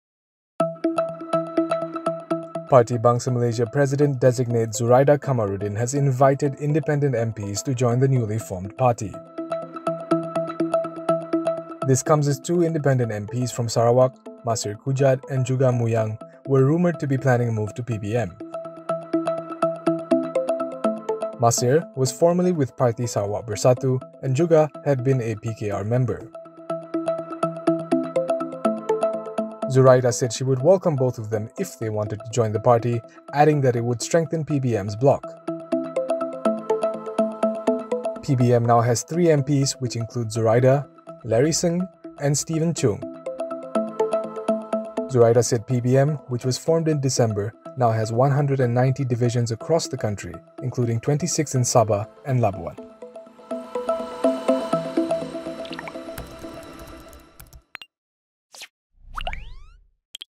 Zuraida invites independent MPs to join PBM
Parti Bangsa Malaysia (PBM) president designate Zuraida Kamaruddin has invited independent MPs to join the newly formed party.